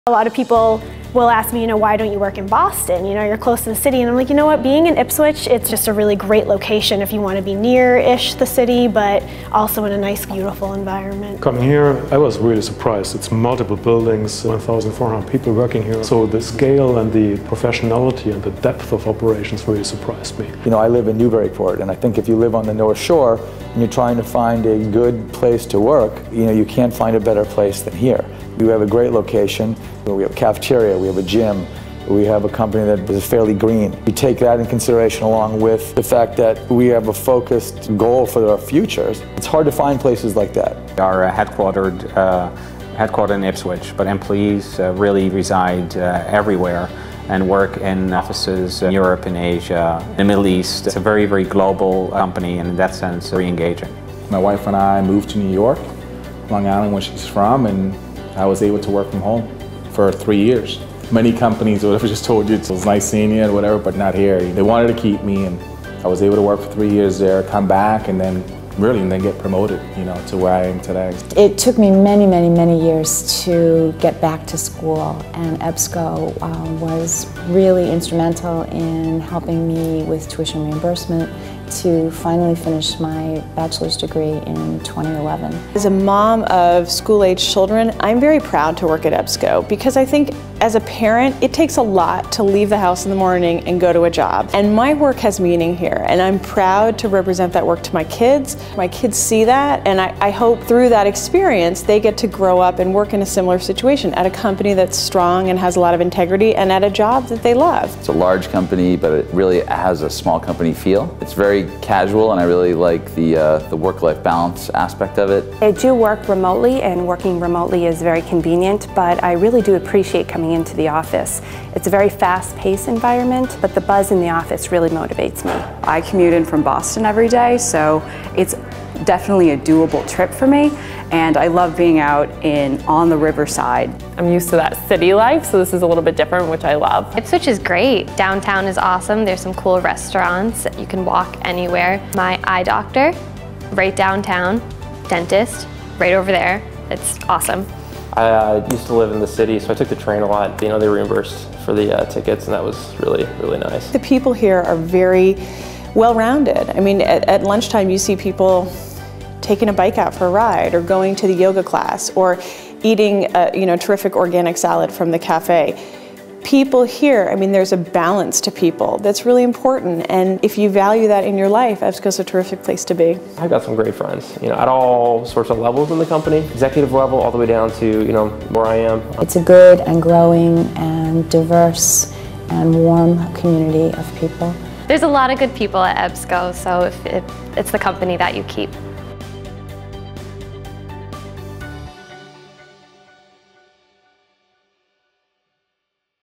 Get to Know EBSCO
Join one of the Fastest-Growing Technology Companies North of Boston

When you join EBSCO, you will be contributing your skills and talents to one of the top research database and eBook providers worldwide. We are the leader in our field due to our cutting-edge technology, forward-thinking philosophy, and top-notch workforce. This is a company that will motivate you, inspire you, and allow you to grow. We are looking for only the best. If you are too, then apply now: